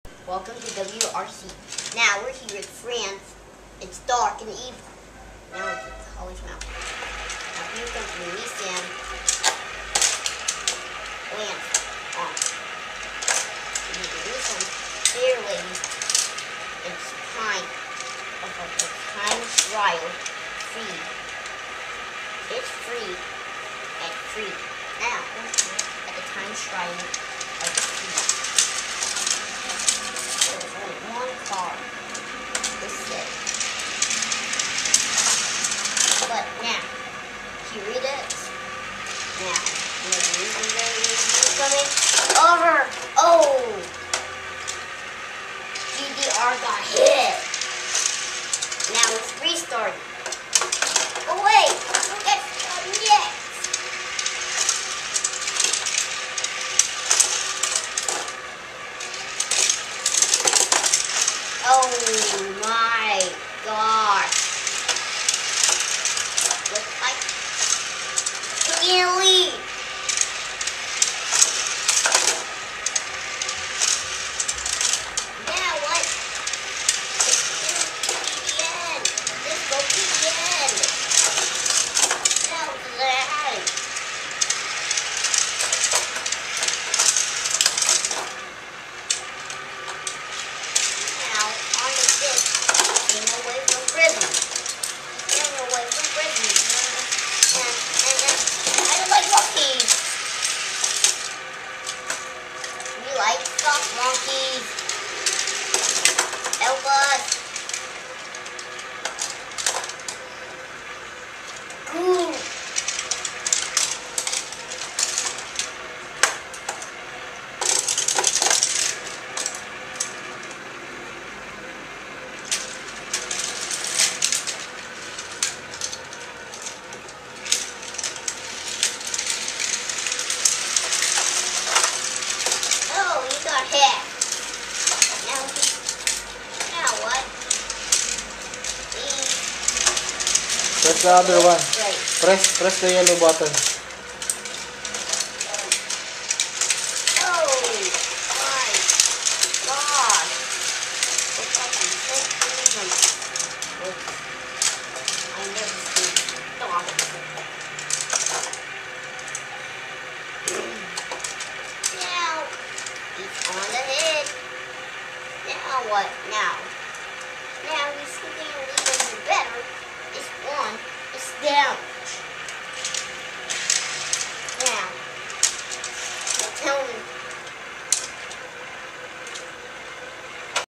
Rocky: WRC Announcer
Rocky just love cars. He collects die-cast cars. He counts the car models & makes we pass by every morning I take him to school. His favorite PSP games are Need For Speed, Gran Turismo & Outrun. In continuation for this love, Rocky said that when he's 35 years old (my age), he will be a WRC race announcer. Hence the dialog ("Welcome to WRC. We are now in France. It's dark & it's evening...") while playing with his Tomica's.